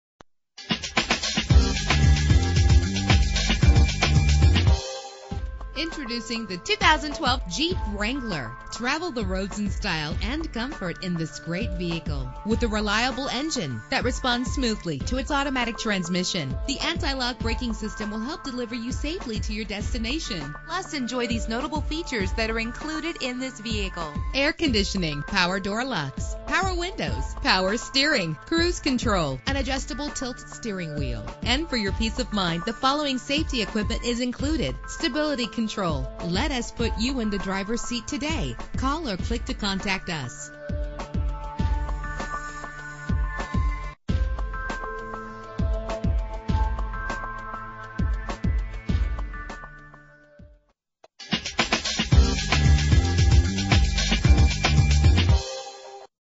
2012 Jeep Wrangler - Carlsbad CA
Year: 2012
Make: Jeep
Model: Wrangler
Trim: Unlimited Sahara Sport Utility 4D
Engine: 6 cylinder 3.6 Liter
Transmission:
Color: Sahara Sand
Mileage: 337
Address:
5555 Car Country Dr.